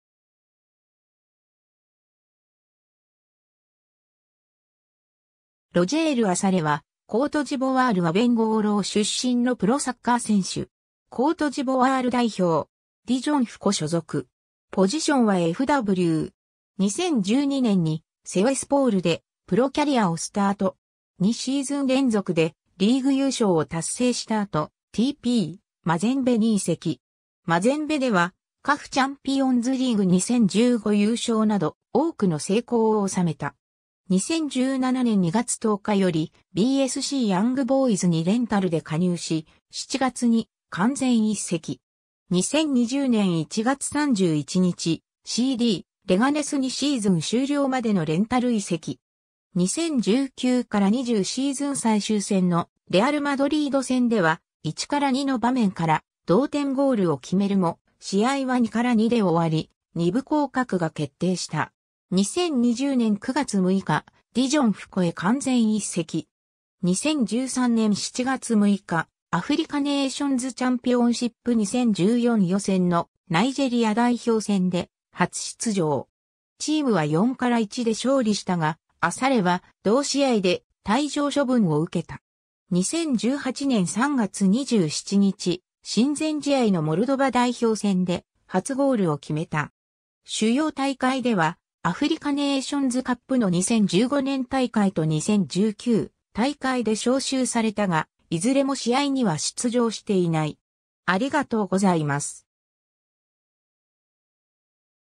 ロジェール・アサレ
ロジェール・アサレ（Roger Assalé、1993年11月13日 - ）は、コートジボワール・アベンゴウロー出身のプロサッカー選手。
コートジボワール代表。
ディジョンFCO所属。
 2012年にセウェ・スポールでプロキャリアをスタート。
2シーズン連続でリーグ優勝を達成した後、TPマゼンベに移籍。
 マゼンベではCAFチャンピオンズリーグ2015優勝など、多くの成功を収めた。
 2017年2月10日よりBSCヤングボーイズにレンタルで加入し、7月に完全移籍。
 2020年1月31日、CDレガネスにシーズン終了までのレンタル移籍。
2019-20シーズン最終戦のレアル・マドリード戦では、1-2の場面から同点ゴールを決めるも、試合は2-2で終わり、2部降格が決定した。
 2020年9月6日、ディジョンFCOへ完全移籍。
 2013年7月6日、アフリカネイションズチャンピオンシップ2014予選のナイジェリア代表戦で初出場。
チームは4-1で勝利したが、アサレは同試合で退場処分を受けた。
2018年3月27日、親善試合のモルドバ代表選で初ゴールを決めた。
 主要大会ではアフリカネイションズカップの2015年大会と2019大会で召集されたが、いずれも試合には出場していない。